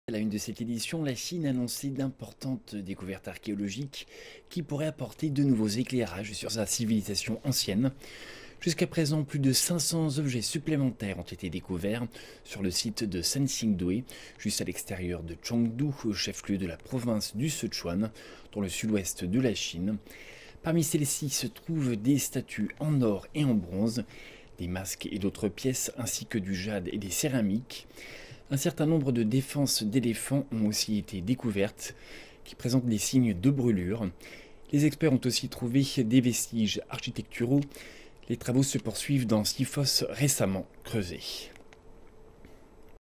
Fouilles en cours de 6 nouvelles fosses dans le Sichuan
La Chine a annoncé d'importantes découvertes archéologiques qui pourraient apporter de nouveaux éclairages sur sa civilisation ancienne. Jusqu'à présent, plus de 500 objets supplémentaires ont été découverts sur le site de Sanxingdui, juste à l'extérieur de Chengdu, chef-lieu de la province du Sichuan, dans le sud-ouest de la Chine. Parmi celles-ci se trouvent des statues en or et en bronze, des masques et d'autres pièces, ainsi que du jade et des céramiques. Un certain nombre de défenses d'éléphants ont aussi été découvertes, qui présentent des signes de brûlures. Les experts ont aussi trouvé des vestiges architecturaux. Les travaux se poursuivent dans six fosses récemment creusées.